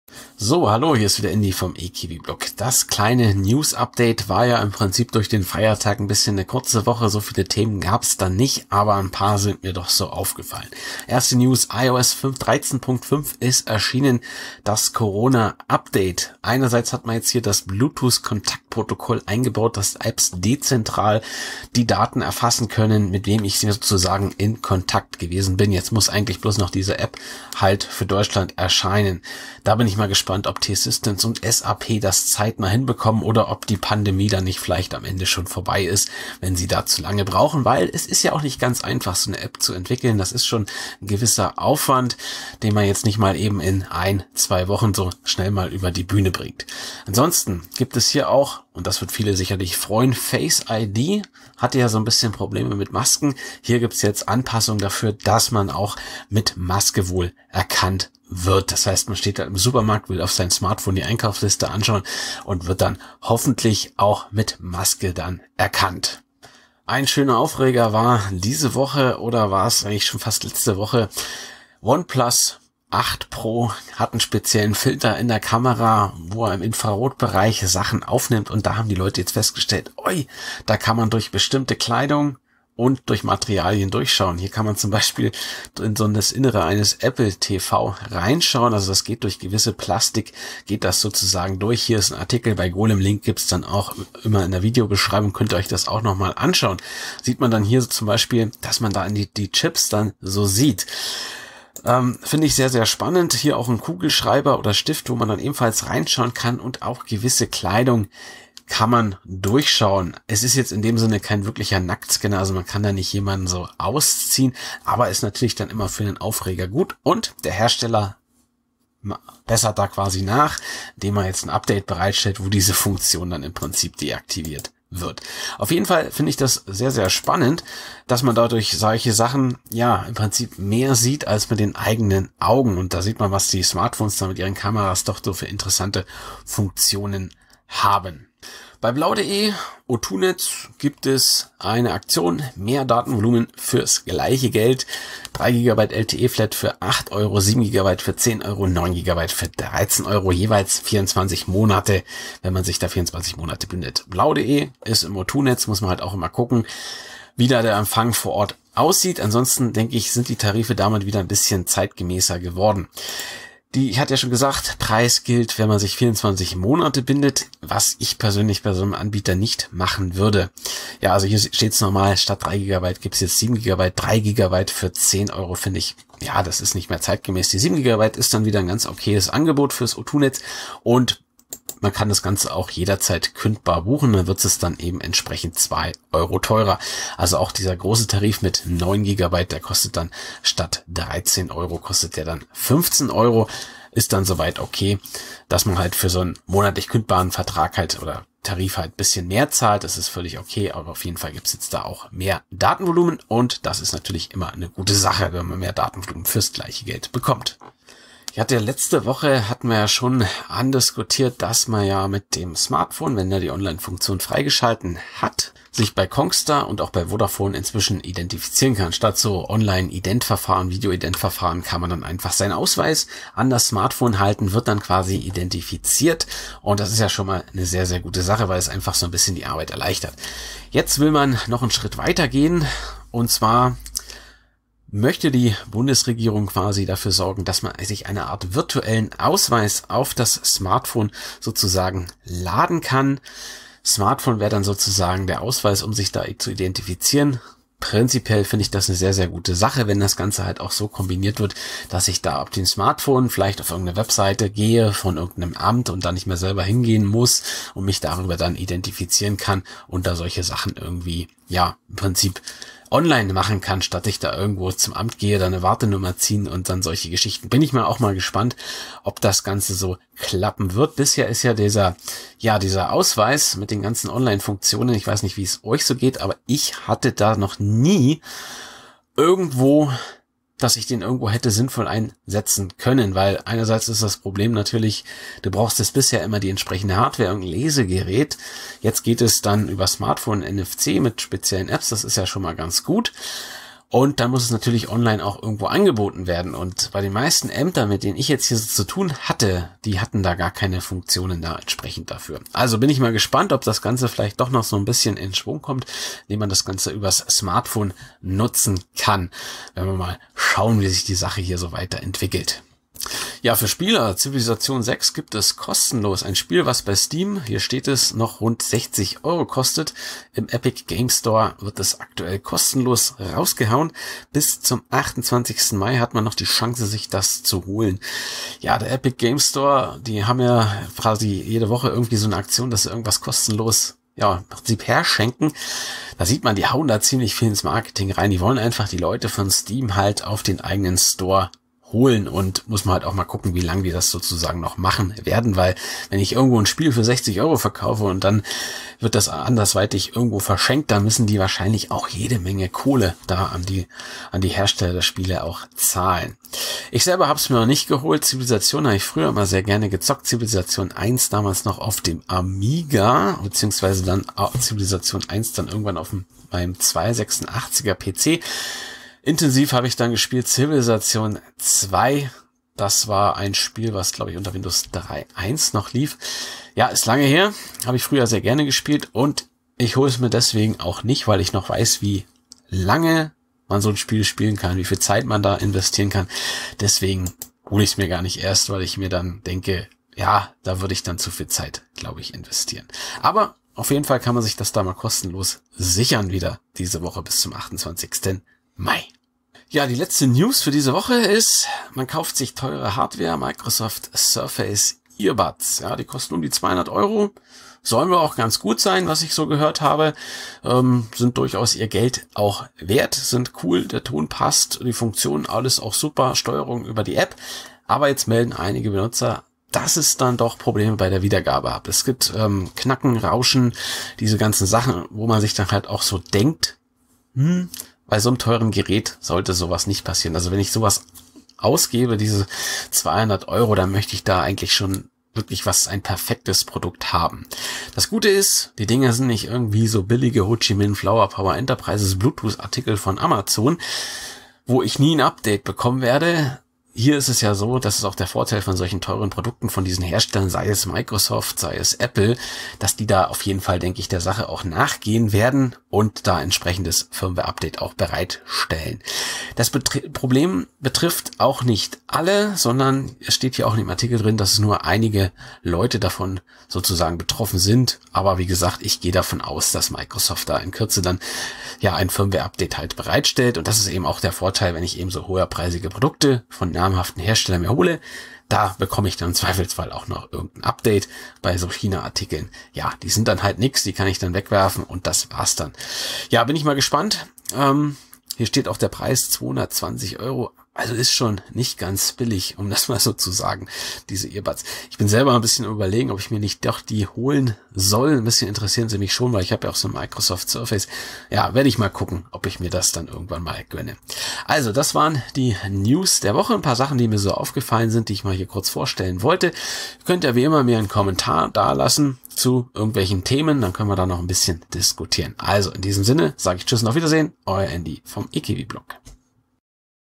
News der Woche 21/2020-Apple Corona Update,Smartphone Röntgenblick,Civ6 kostenlos,Blau.de,EarBuds
Microsoft hat Probleme mit seinen Earbuds, Rauschen und Knacken, darüber klagen ein paar Nutzer, OnePlus hat einen kleinen Skandal an der Backe, mit dem Infrarotfilter kann man nicht nur in Apple-TVs hinein schauen, sondern auch durch gewisse Kleidung. Apple bereitet IOS für die Corona-App vor und Face-ID kann nun auch besser mit Gesichtsmasken umgehen. Noch zuviel Zeit? Civilization 6 gibt es derzeit kostenlos im Epic Game Store. Der Perso soll virtuell aufs Smartphone, schauen wir mal ob das was wird.